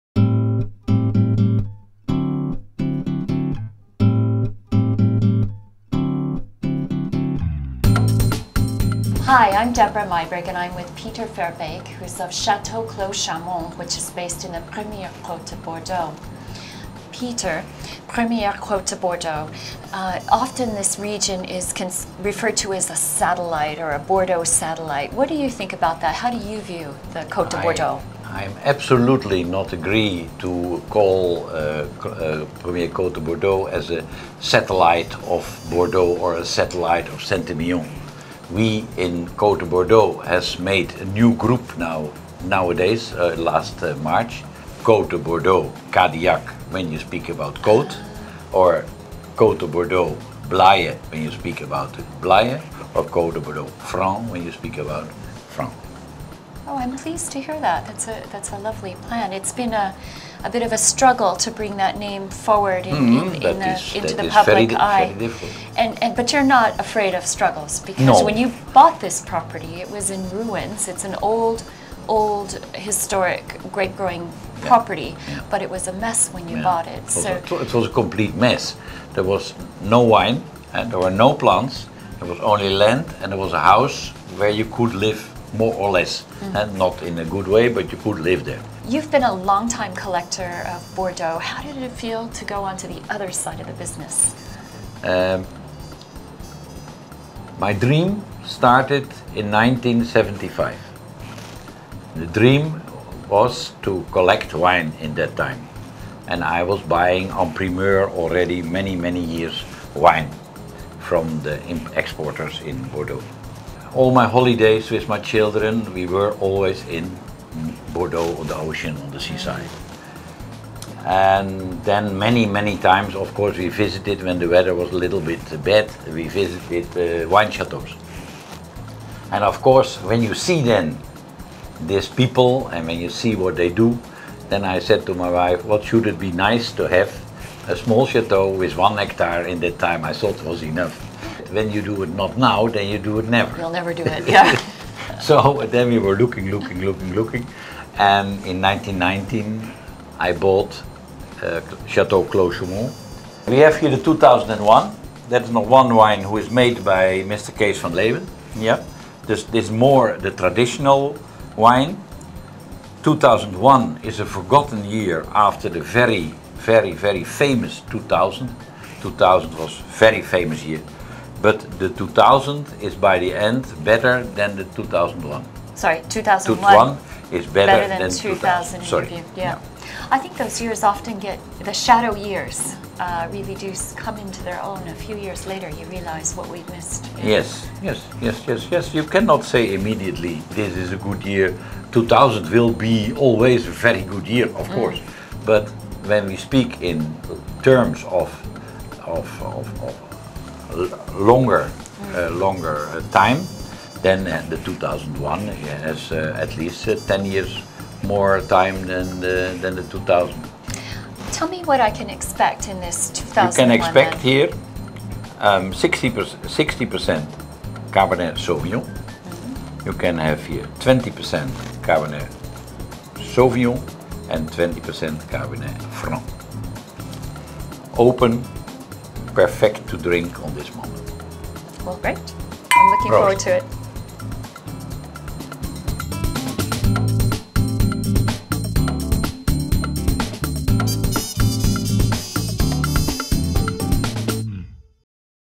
Meet the Winemaker (Episode 26): Pieter Verbeek, Château Clos Chaumont, by Debra Meiburg MW
Pieter Verbeek of Chateau Clos Chaumont, based in Premier Cote du Bordeaux, which is often referred to as a satellite of Bordeaux. Pieter strongly disagrees! Pieter and Debra discuss how he brought the property in poor conditions with no wine or plants and a dishevelled house, and transformed it to the state it is today. As a former collector with a passion for Bordeaux, Pieter tells how he is living his dream by crossing over into the industry.  Pieter shares why he thinks 2001 is actually better than the standout 2000.